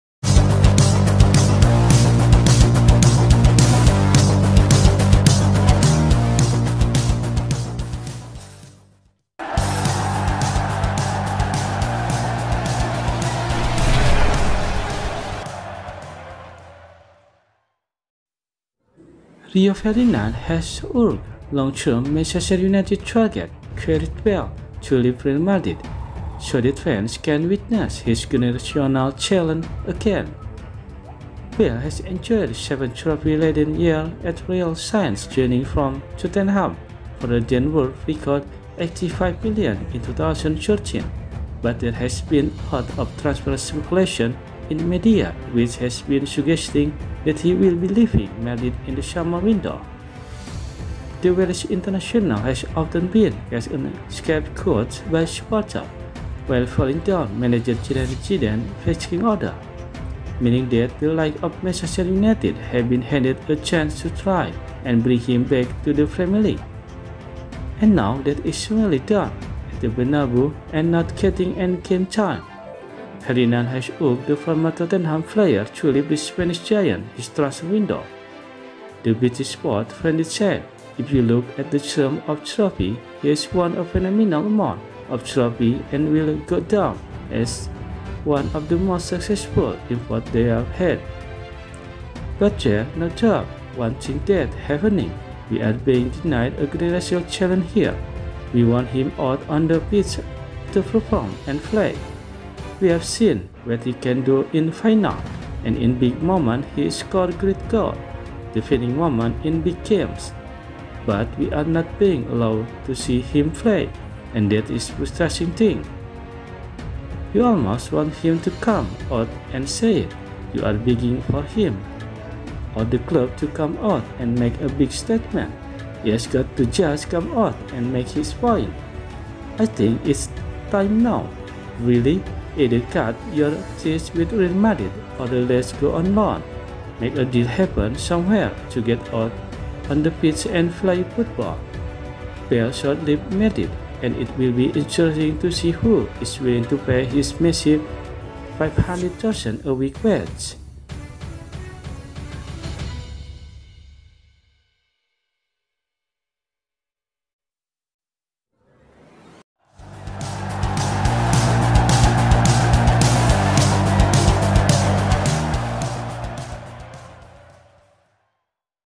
Ferdinand agree superstar Manchester United target attacker to leave Real Madrid
Rio Ferdinand has urged long-term Manchester United target Gareth Bale to leave Real Madrid, so that fans can witness his “generational talent” again.

Bale has enjoyed seven trophy-laden years at Real since joining from Tottenham for a then world-record £85m in 2013, but there has been alot of transfer speculation in the media which has been suggesting that he will be leaving Madrid in the summer window.